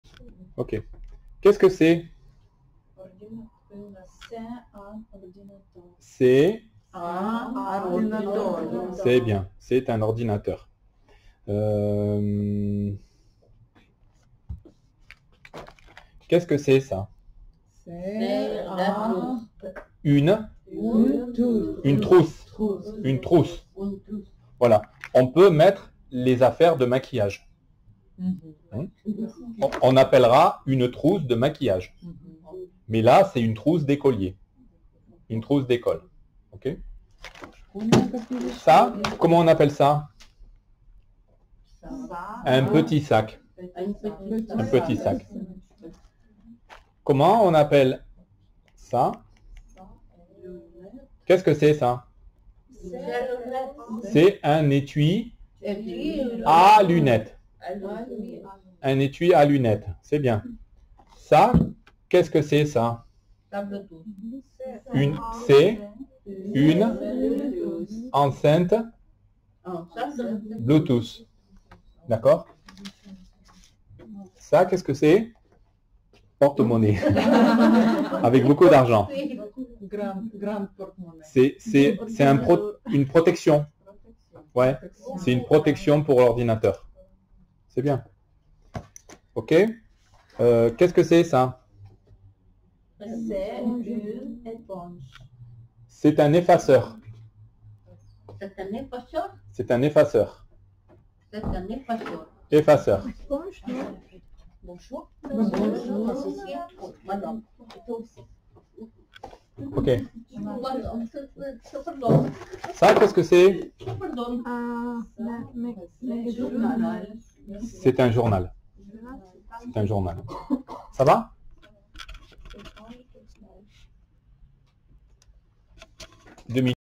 INFRA A1.1
Le présentatif ''c'est''
la question : qu'est-ce que c'est ?